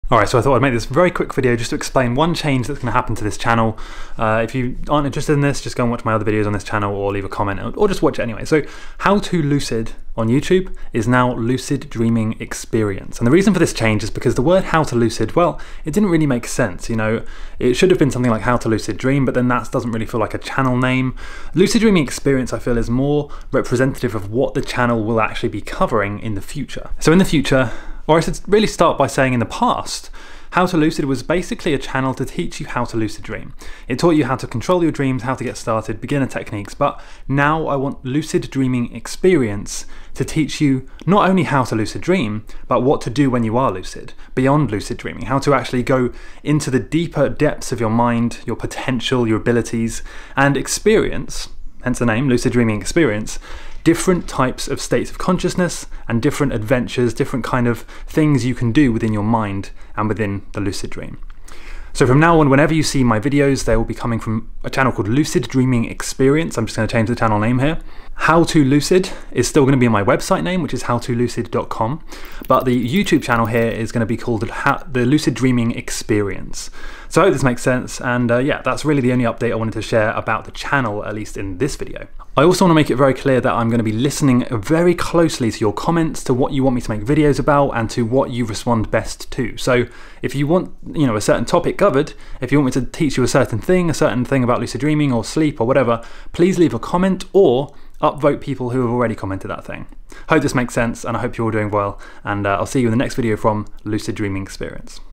HowToLucid Is Now 'Lucid Dreaming Experience'
✨ My channel HowtoLucid is now going to be called 'Lucid Dreaming Experience' which reflects the content and style of videos I'll be making in the future. HowToLucid was focused on teaching people HOW to lucid dream, but lucid dreaming experience is more about BEYOND lucid dreaming, and what you can do when you LEARN to lucid dream.

I'll be going beyond the basics, and into advanced stuff but I will ALSO make beginner tutorials as well. Lucid Dreaming Experience feels like a much more professional and relevant name, so from now on that's what this channel is called.